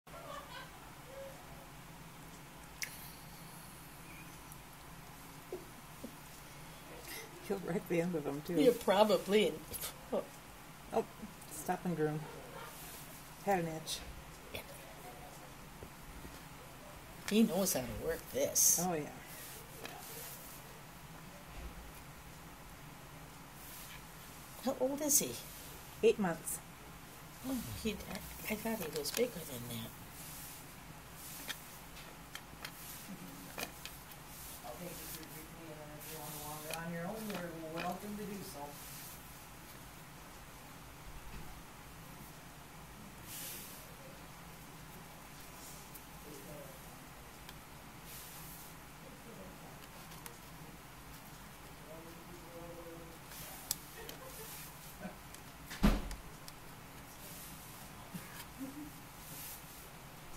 Shoe laces are one of Corkys favorite things to play with
Corky visited the shelter during the open house and enjoyed some playtime with one of the volunteers shoes.